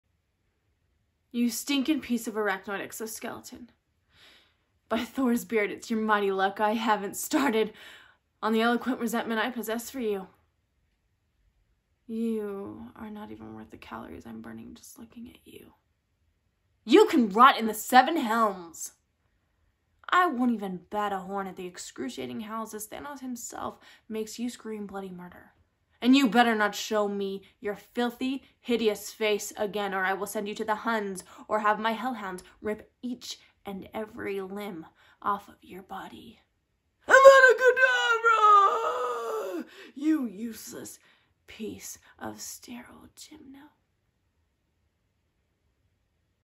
“Nerd” Monologue
🚨🚨Found this one on Pinterest of all places! 🚨🚨This could definitely be done as a more stereotypical nerd style, but I chose not to do that. My current regret is not adding more of an element of a nerd within the monologue though. In my movement or way of speech, but just keeping it subtle still.

Monologue:
“You stinking piece or arachnoid exoskeleton, by Thor’s beard, it’s your mighty luck I haven’t started on the eloquent resentment I possess for you. You are not even worth the calories I am burning even looking at you. You can go rot in the seven helms. I would not even bat a horn at those excruciating howls while Thanos himself makes you scream bloody murder. You better not show me your filthy hideous face or I won’t hesitate to throw you to the Huns or maybe my hell hounds to rip each and every limb apart. Avada kedavra you useless piece of sterile gymno.”